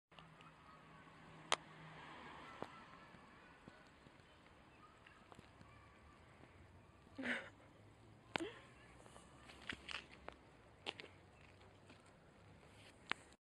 Chceme sníh !
Natočila jsem jak Vánoční stromeček visí na okením věšáku :D
a Náš první sníh který není :( - Autor: Petra Zimova